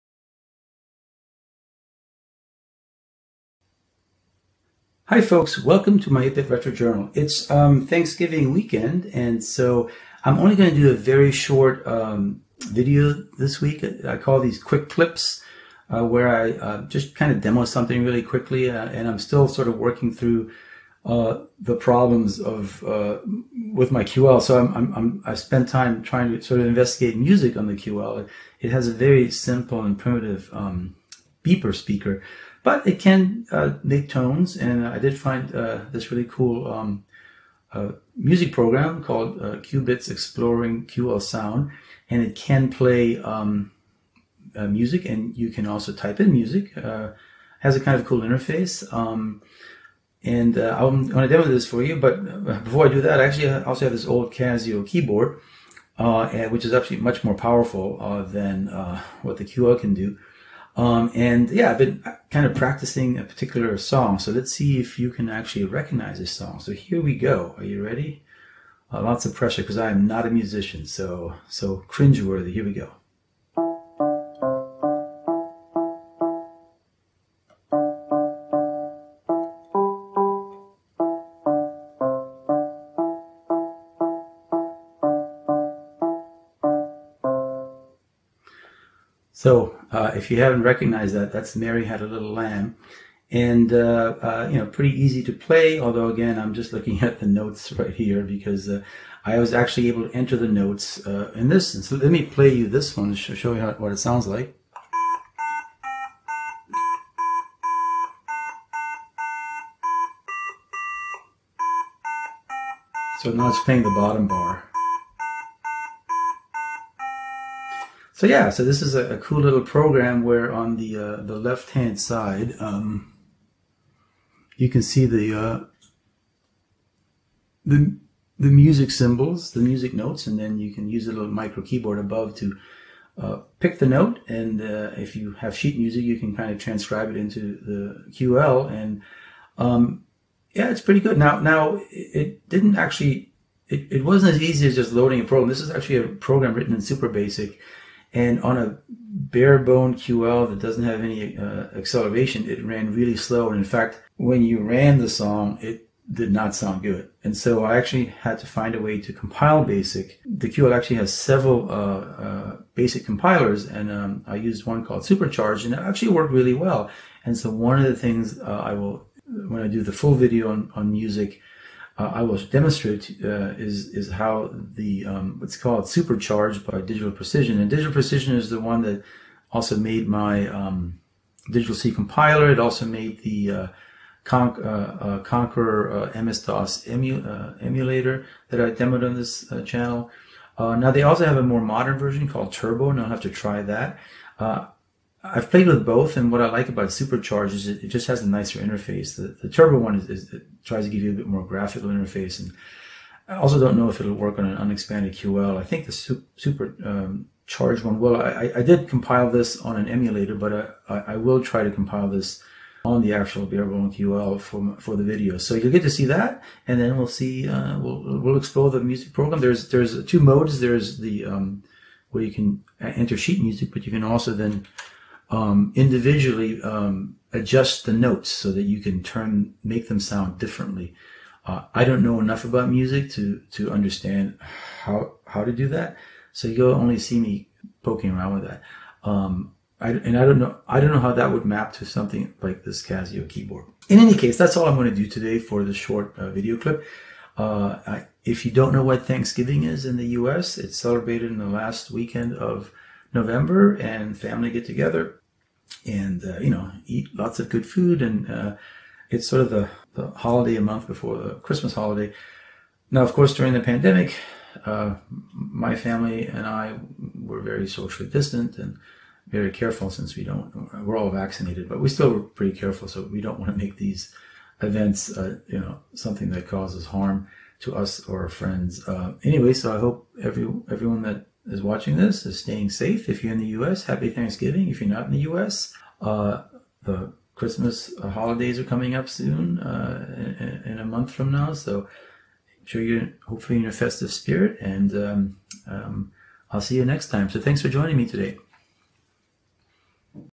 Quick Clip: Sinclair QL Playing a Nursery Rhyme
Just a quick clip about what I've been up to during the Thanksgiving holidays. I found a nifty music program, QBITS Exploring QL Sound, written completely in SuperBASIC. I'm not a musician but still had fun with it. See it play a children's classic.